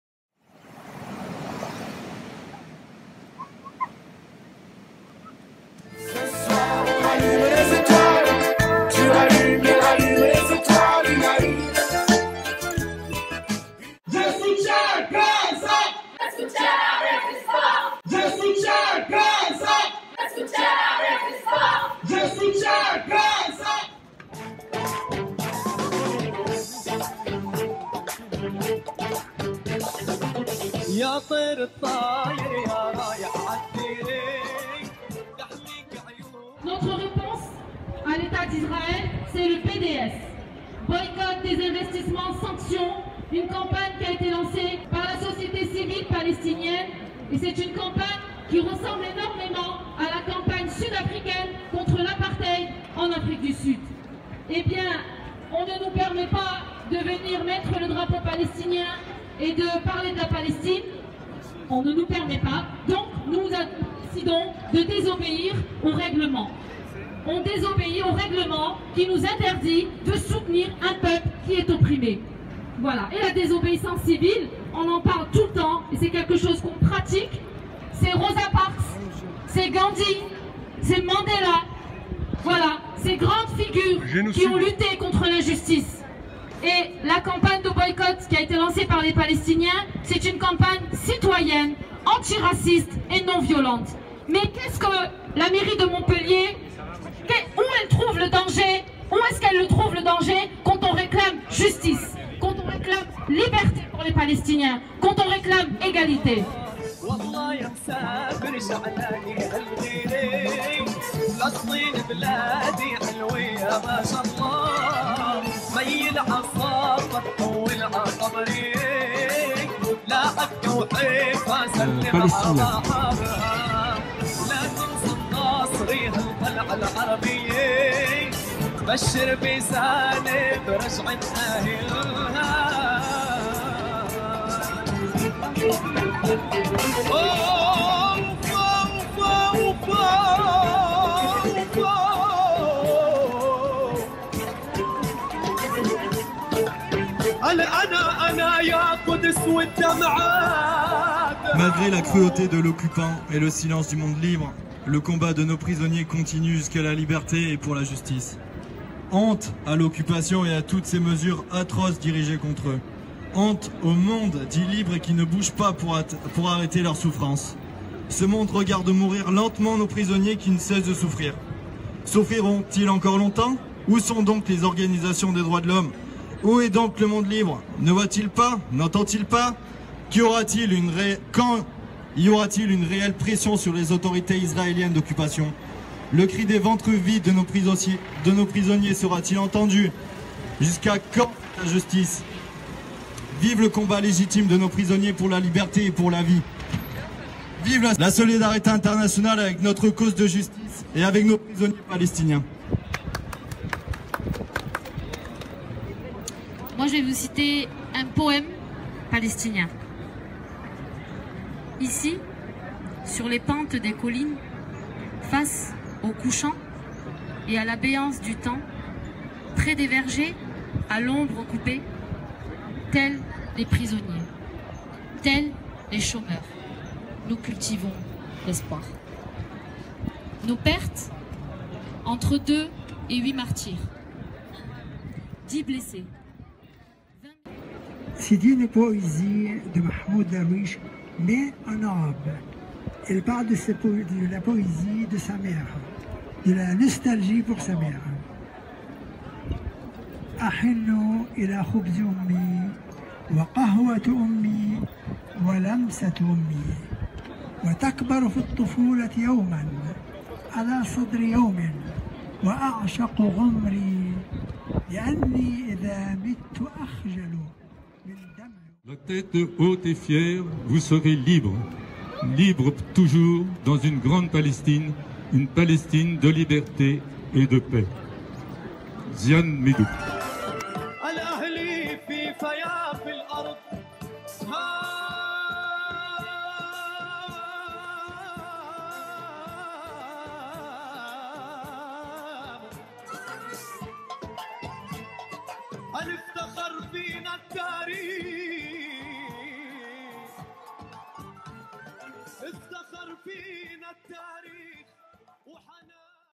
Montpellier : Pour les prisonniers Palestiniens.
Samedi 21 avril la Campagne BDS France Montpellier tenait-malgré l'interdiction de la police- son stand de solidarité avec la Grande Marche pour le Retour. L'action est dédiée aux prisonnier Palestiniens.